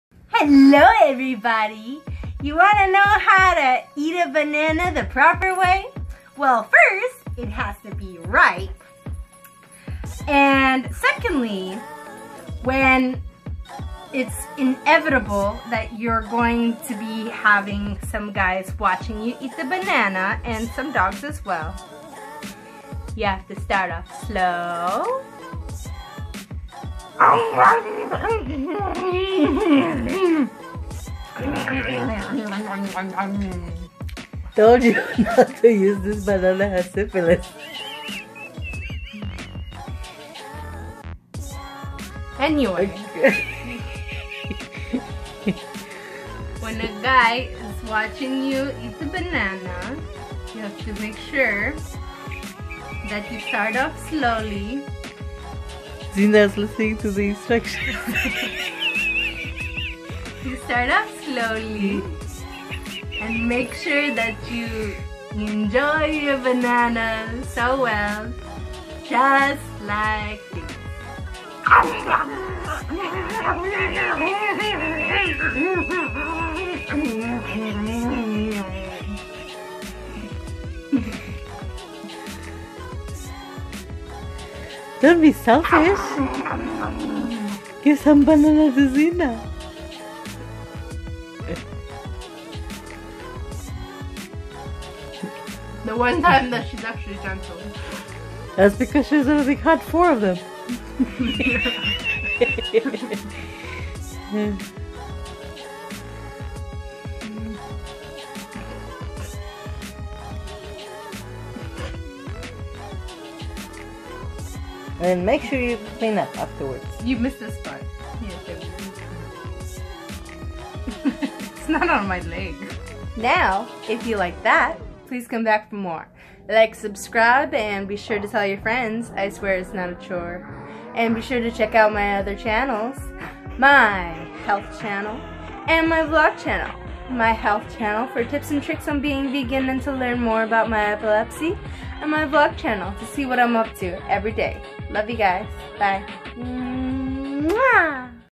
Eating Bananas With An Audience
We've all had those days when we just couldn't avoid someone watching us eat a banana when we just wanted to have time to chill out, but I'm here to help you deal with those banana creeps! 😊